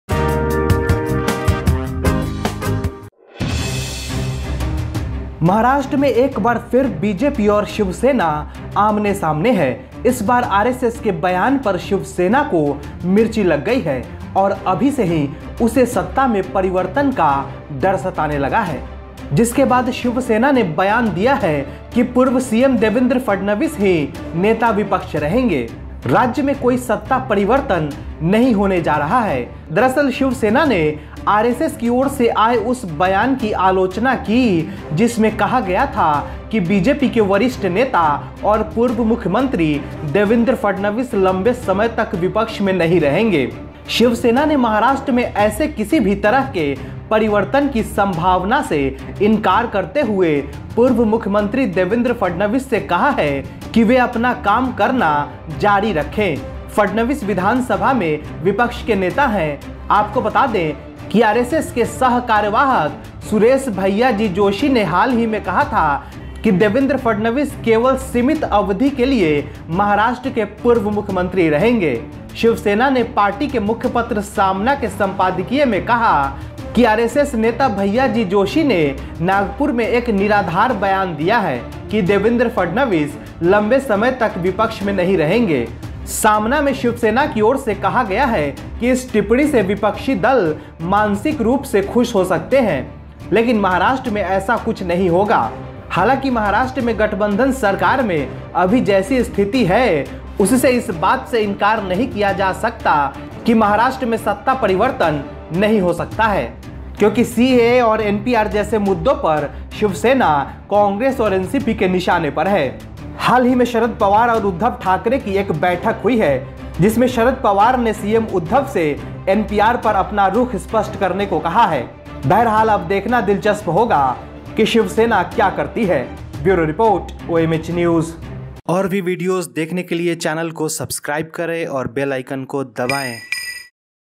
Shiv Sena's big statement on RSS
BJP Shiv Sena is face to face once again in Maharashtra. This time the Shiv Sena has got chilly on the statement of the ASS and has already started to fear the change of power. After which the Shiv Sena has given a statement that only former Chief Minister Devendra Fandavasi will remain the opposition. There is no change of power in the state. In fact, Shiv Sena criticized the statement from RSS, which said that senior BJP leader and former Chief Minister Devendra Fadnavis will not remain in opposition for long. Shiv Sena has ruled out the possibility of any such change in Maharashtra and asked former Chief Minister Devendra Fadnavis to continue his work. Fadnavis is the Leader of the Opposition in the Assembly. Let us tell you that Suresh Bhaiyyaji Joshi of Sarkaryavah of RSS recently said that Devendra Fadnavis will be the former Chief Minister of Maharashtra for a limited period only. Shiv Sena said in the editorial of Saamana that Bhaiyaji Joshi has given a statement that Devendra Fadnavis will not remain in opposition for long.